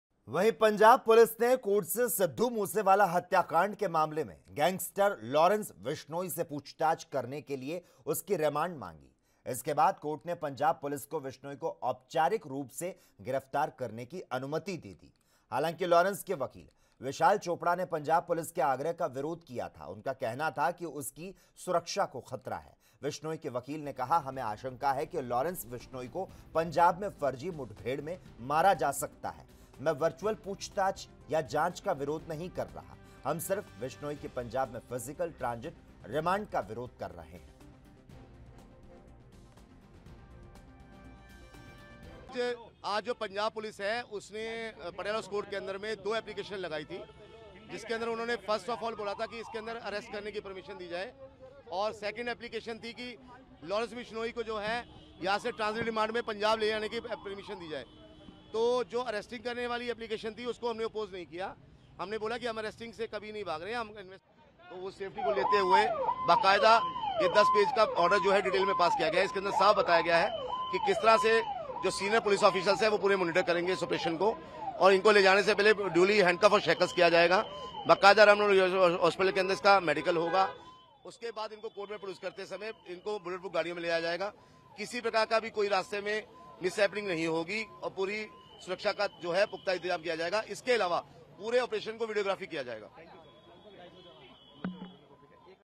Sidhu MooseWala हत्याकांड मामला' Lawrence Bishnoi से पूछताछ करने के लिए मांगी रिमांड | ANB NEWS
सिद्धू मूसेवाला हत्याकांड मामला
लॉरेंस बिश्नोई से पूछताछ करने के लिए मांगी रिमांड
पंजाब पुलिस ने पूछताछ के लिए मांगी थी रिमांड
कोर्ट ने गिरफ्तार करने की दी अनुमति